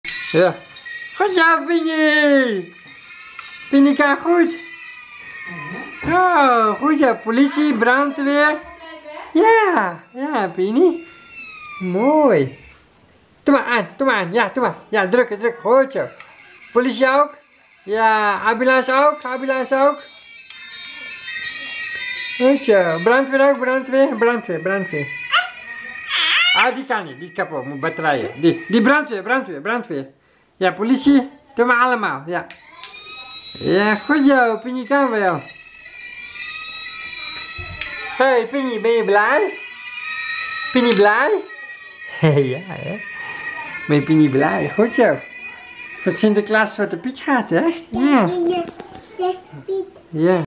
Pini sinterklaas kadootje gekregen....
Ja .somige Sinterklaas kadootjes gaan maar 1 dag mee. Pini moet nu zijn kadootje repareren samenmet Dady.